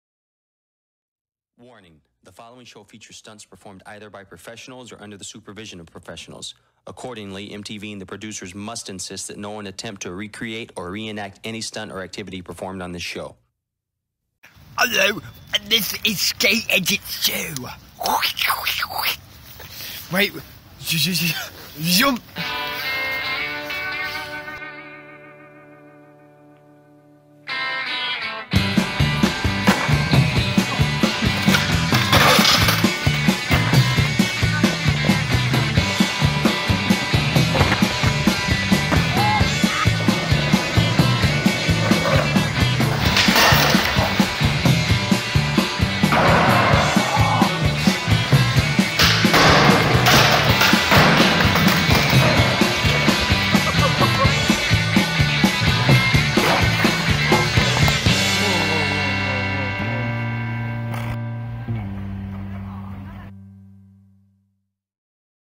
Skate edit 2 - trailer
Actually learned how to use vegas pro so quality should be better now. I haven't gotten round to editing the whole thing because I'm a lazy shit so have this trailer for the time being yah dig.